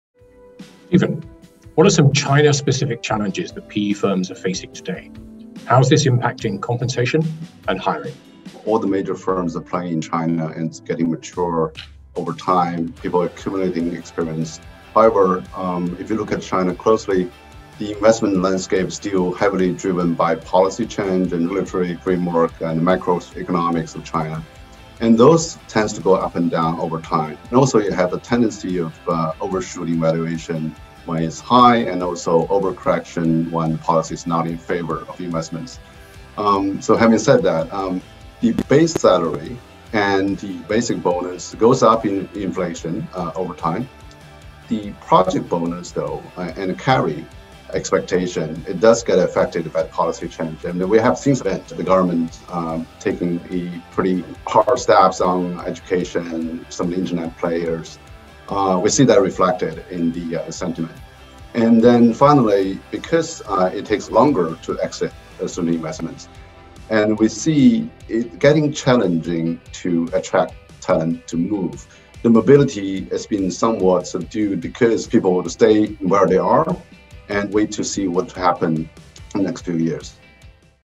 Private equity challenges and implications on hiring in China
What challenges are PE firms facing today in China and the implications on compensation and hiring of investment professionals? Find out in this video of our partner Stephen Zhang.